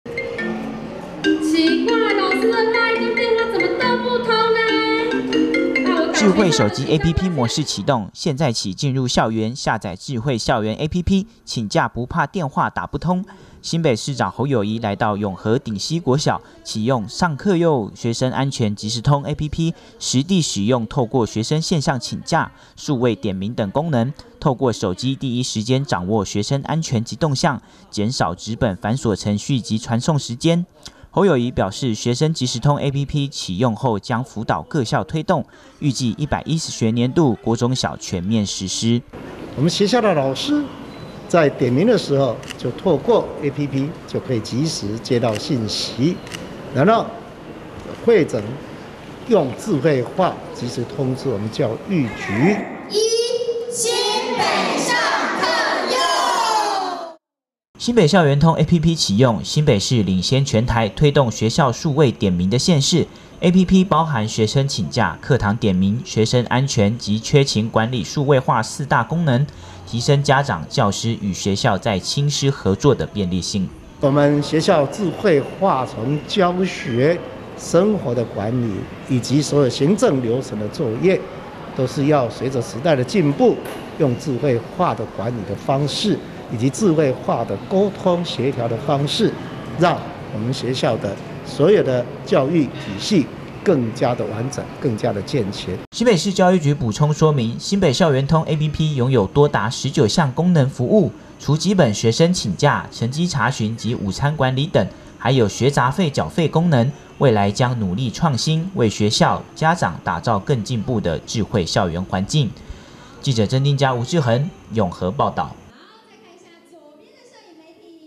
新北智慧校園啟用 校園APP真方便
新北首創數位點名讓智慧生活融入校園，讓學生家長與校方不再為學生臨時請假及點名大傷腦筋，市長侯友宜18日前往永和頂溪國小啟用「上課yo!學生安全即時通」系統，落實行政減量，讓孩童家長既省時又方便。